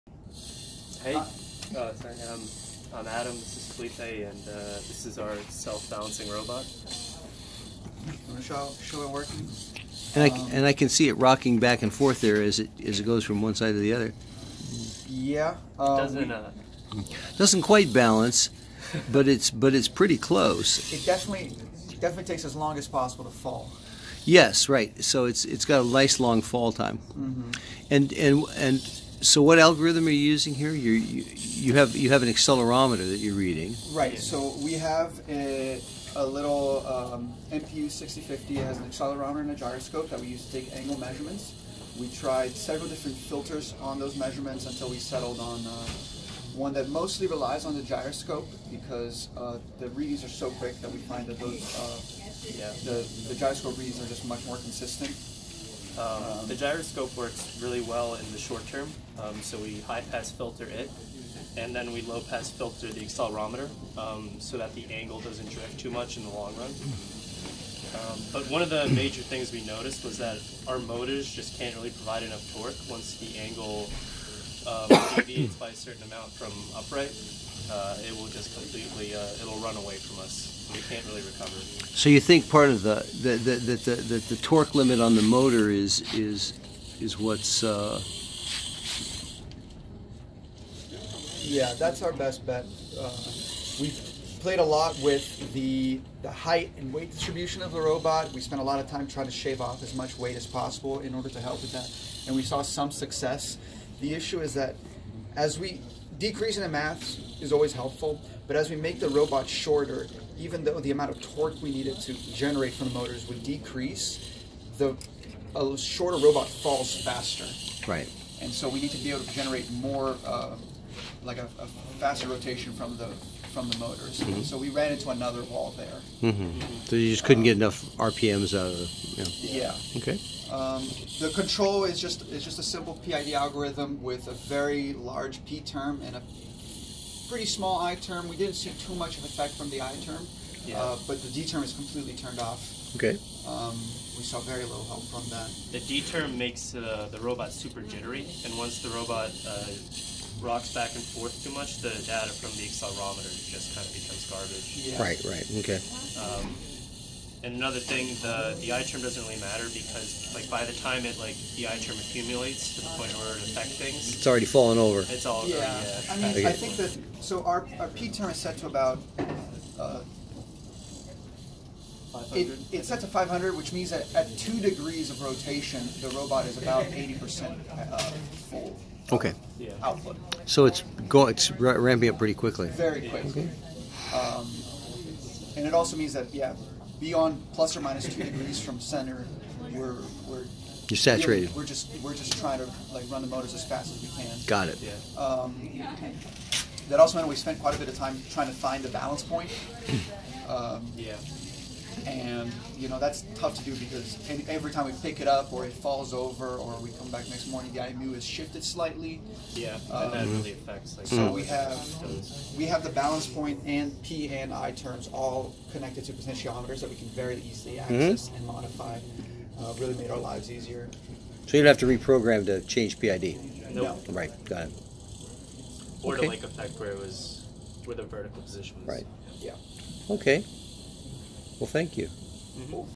BalanceBot
Our project for the course ECE 4760 was to build our own self-balancing two-wheeled robot. Our robot is controlled by a PIC32 microcontroller, uses an inertial measurement unit to keep track of its pitch value and two motors to drive the wheels at the bottom of the robot, and it attempts to maintain equilibrium by using a PID controller to counteract its pitch value by driving the wheels such that they move beneath the center of mass. Working on this project allowed us to learn about the many challenges involved in controlling an unstable system, about how to efficiently debug a system by using a mixture of study and experimentation, and about the principles behind many good filtering systems that we used to filter data from our inertial measurement unit.